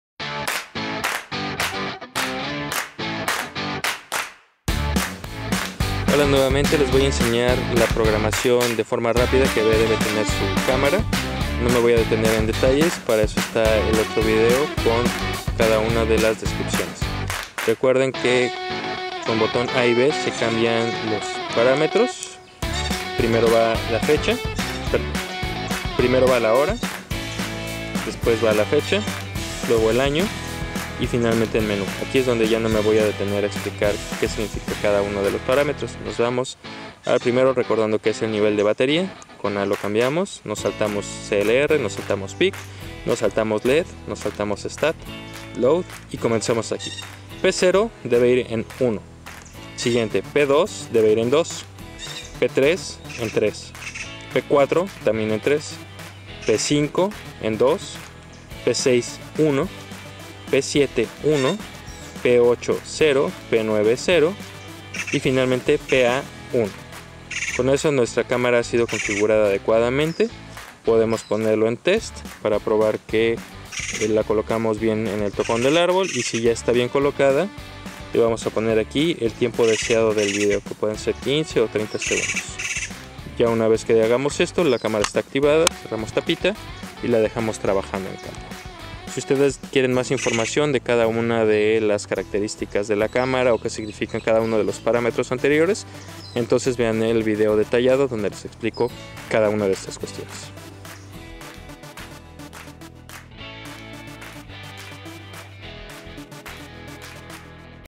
Programación Rápida de la Cuddeback IR modelo H-1453
En este video les mostramos solamente los parámetros qeu deben configurar en su cámara Cuddeback IR H-1453.